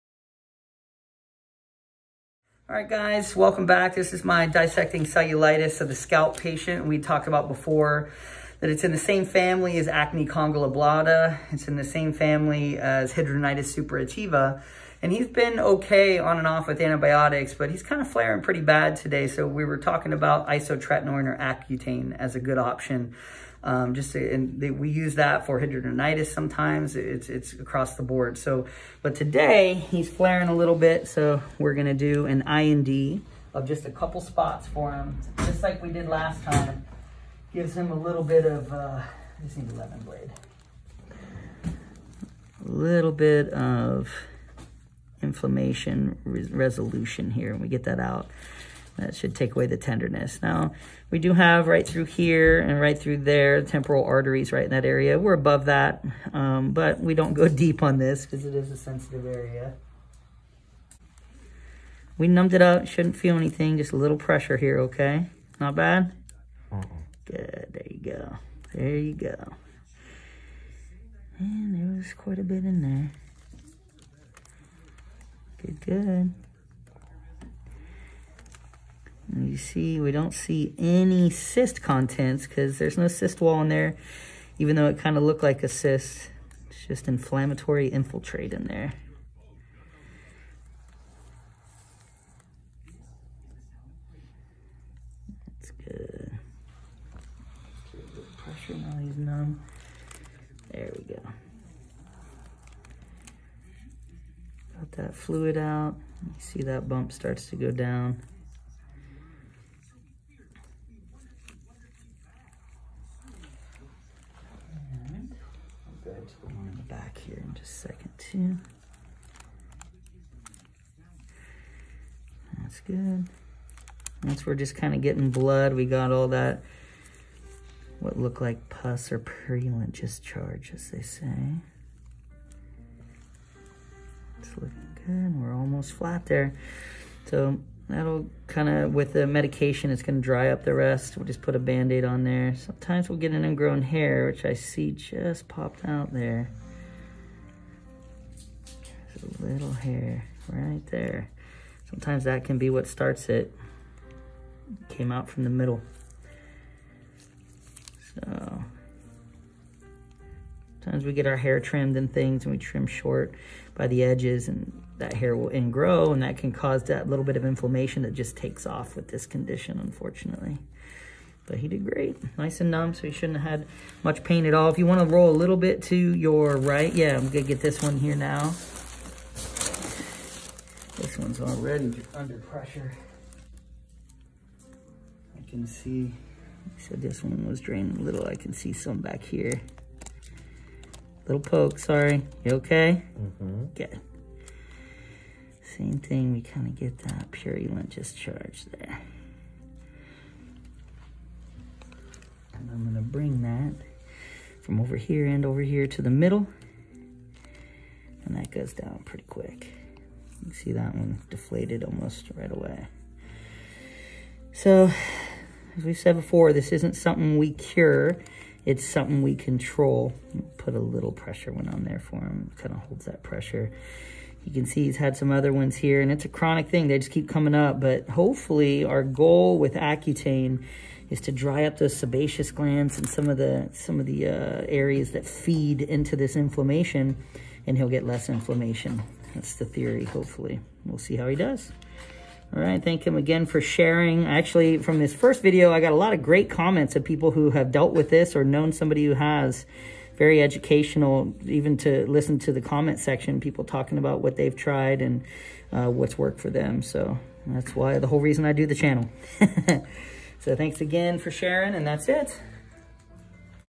Dissecting cellulitis of the scalp. I+D of pus pockets. Drainage of purulent discharge. MrPopZit
Dissecting cellulitis of the scalp follow up. My patient was doing great for many months on intermittent antibiotics and topicals. However in the last several weeks he started flaring with many more tender nodules popping up on the scalp. This condition can be very tricky and flare unexpectedly. We are going to start a course of Accutane to see if this improves his condition.  We have several other treatment options if Accutane does not stabilize the inflammation. We will follow up regularly until he is stable. Pus pocket drainage. These are not cysts so you will not see cyst particles or cyst wall. MrPopZit